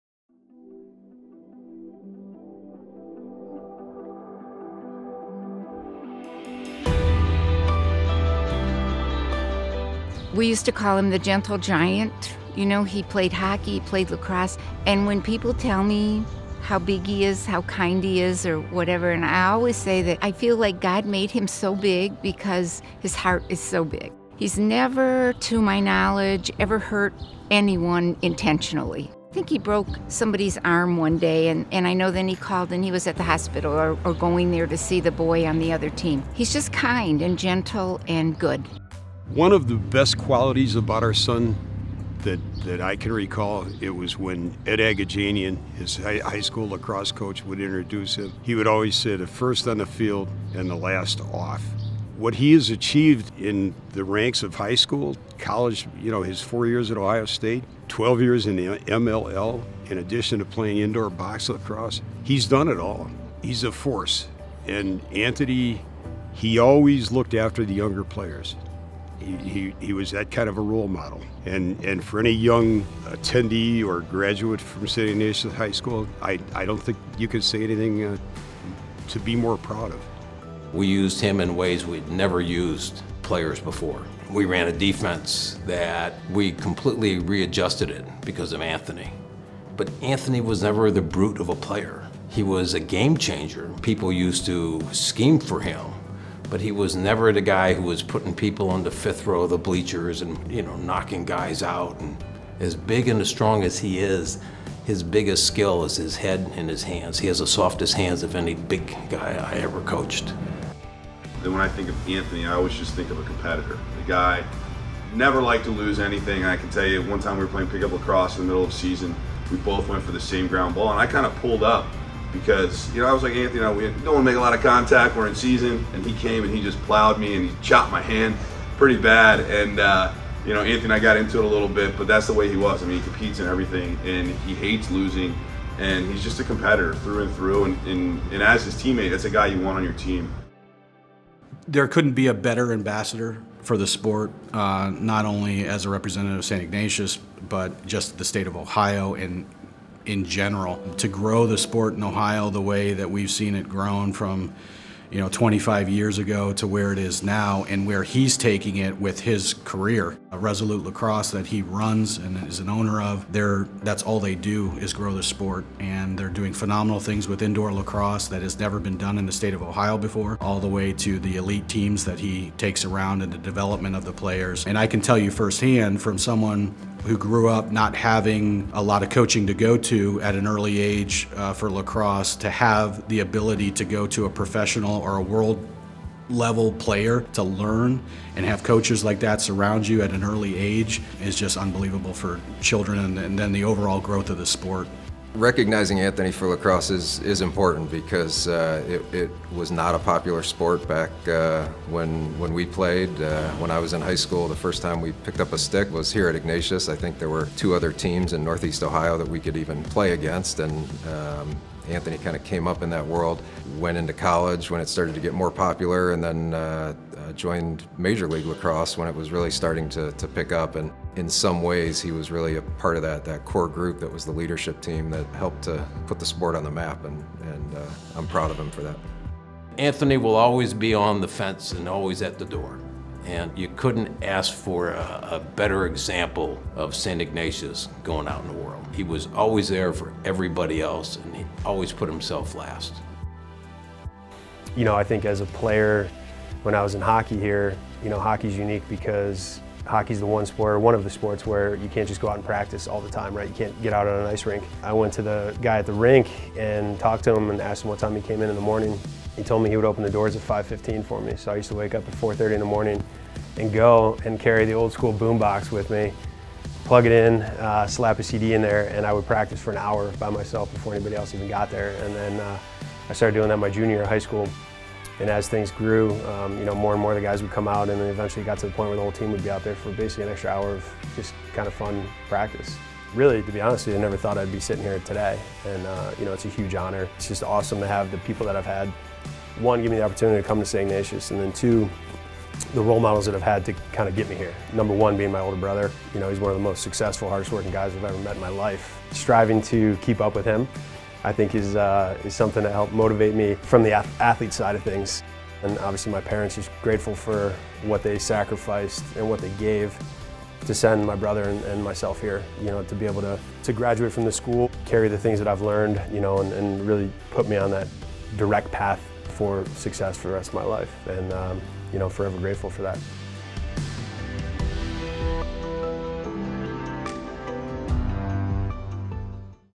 2019 Athletic Hall of Fame: Anthony Kelly '99
Anthony Kelly ’99 is a lacrosse evangelist. After 17 seasons on professional teams, he co-founded Resolute Lacrosse to train and develop young players and inspire a passion for lacrosse in the Midwest. Anthony is the first lacrosse player ever inducted into the Saint Ignatius Athletic Hall of Fame.